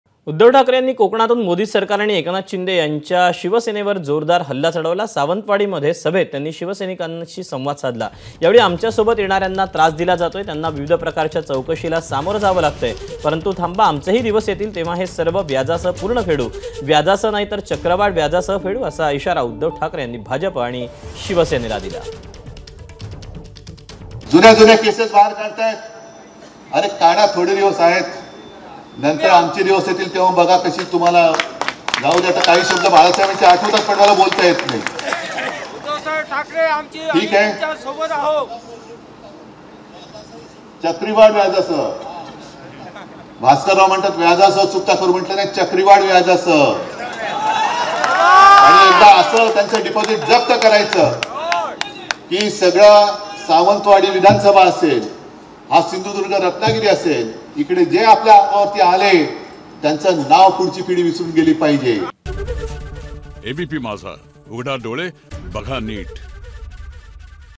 Uddhav Thackeray : आमचे दिवस आल्यावर व्याजासकट पूर्ण घेऊ - उद्धव ठाकरे
Uddhav Thackeray : आमचे दिवस आल्यावर व्याजासकट पूर्ण घेऊ - उद्धव ठाकरे गद्दारांच्या नाकावर टिच्चून शिवसेनेचा मुख्यमंत्री करुन दाखवणारच, मालवणमधून उद्धव ठाकरेंचा भाजपसह शिंदे गटाला इशारा, मतांसाठी मोदींना शिवरायांची आठवण आली, ठाकरेंची टीका.